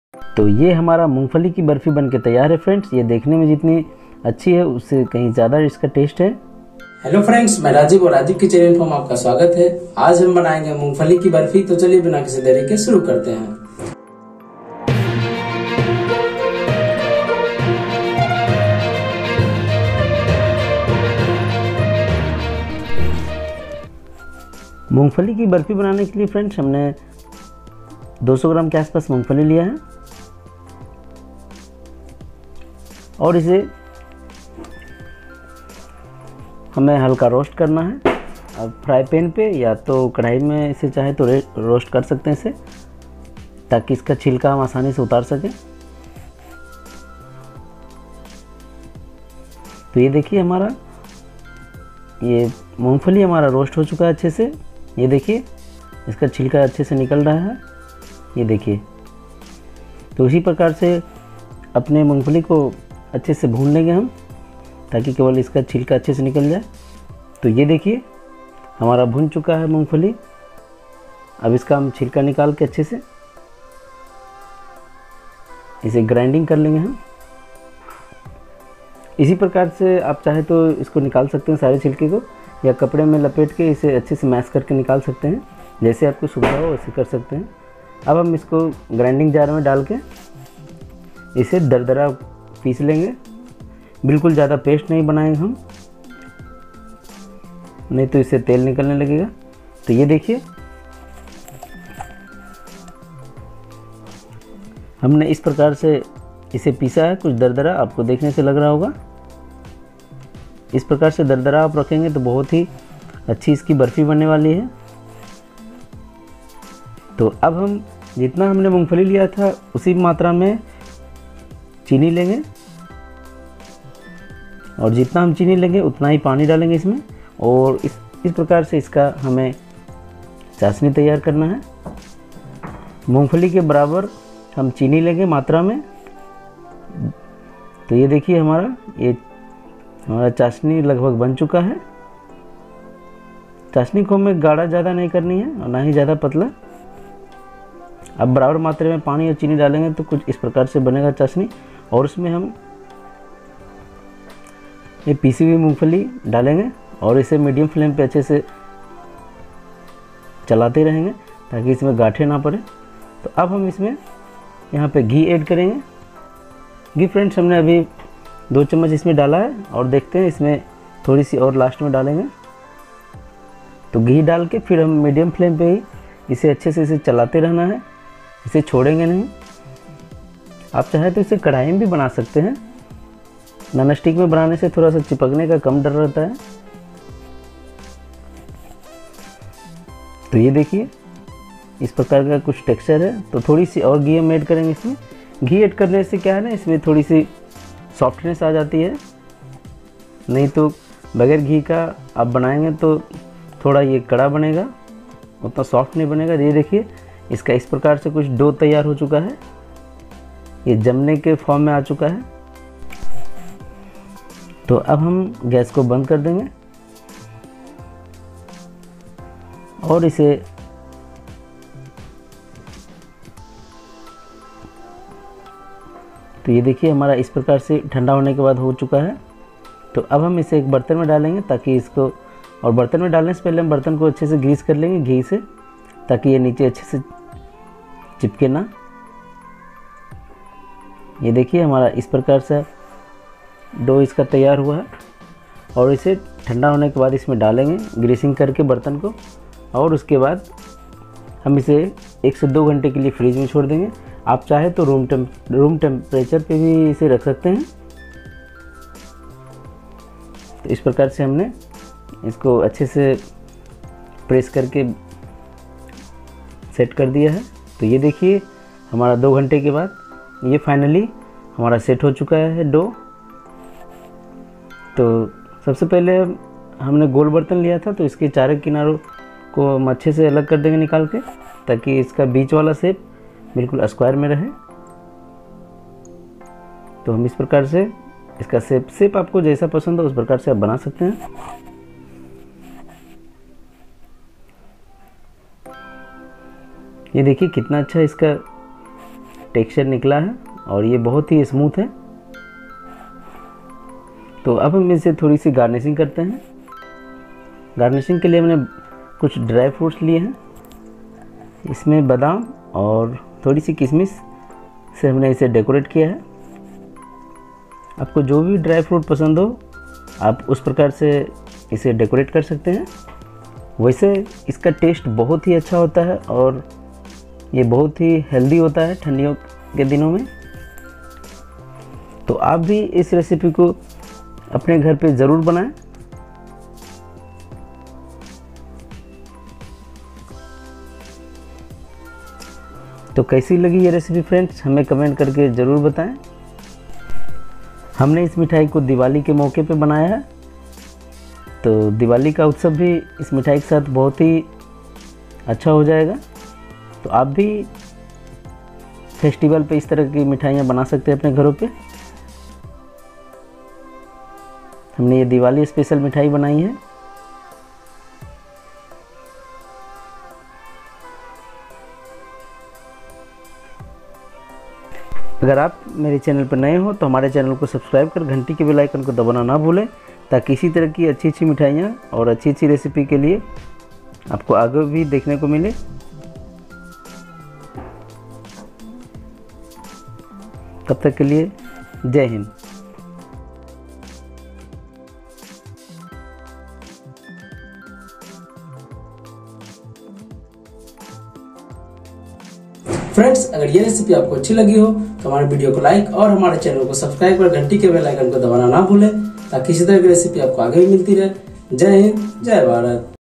बाजार से मिठाई लाने की जरूरत नहीं इस दीवाली बनायें मूंगफली बर्फी|Mungfali barfi recipe
Hello friends aj hamne MUNGFALI BARFI KI recipe banaya hai jo ki bht hi tasty aur aasan tarike se bilkul ban bhi jata hai to aap bhi es recipe ko try jarur karein... Aur hame batayein ki re recipe aapko kaisi lagi...

THANKS. Mungfali barfi, mungfali ki barfi, mungfali ki mithai, mungfali recipe,